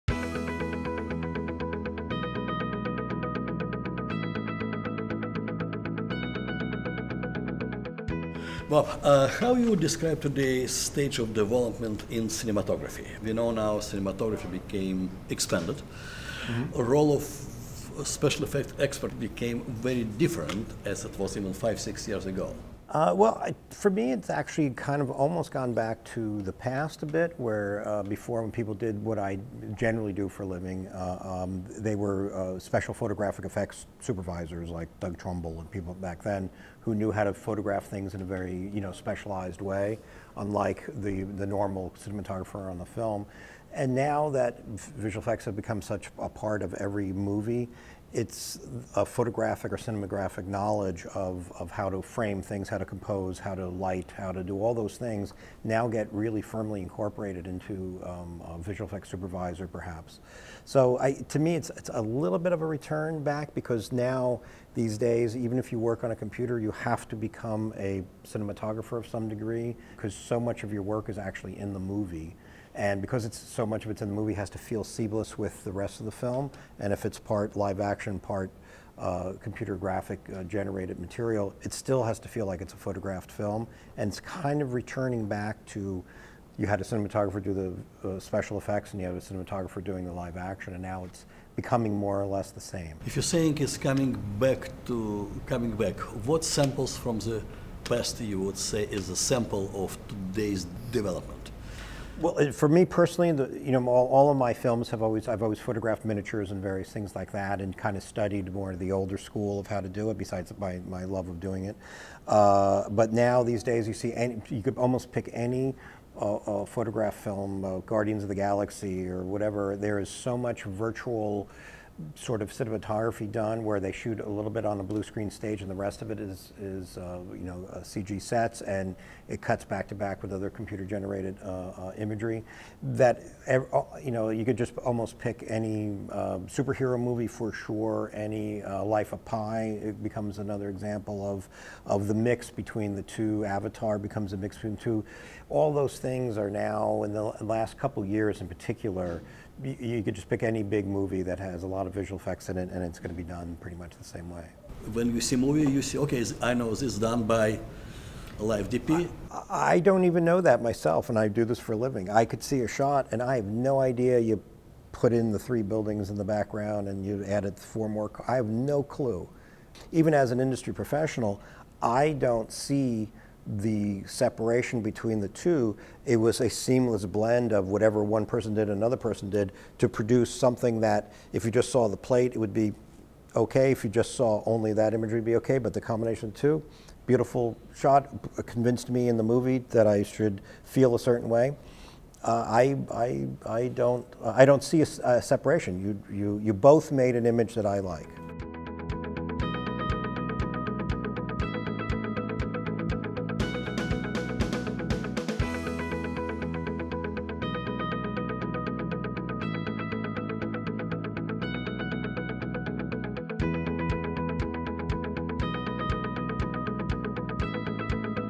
"Evolution of VFX and Cinematography" with Robert Legato, ASC
Two-time VFX Oscar Winner (Hugo - 2011, Titanic - 1997), Director of Photography and VFX Supervisor Robert Legato, ASC sits down with GCI Co-Founder Yuri Neyman, ASC to discuss how VFX has changed the way Cinematographers approach visual storytelling.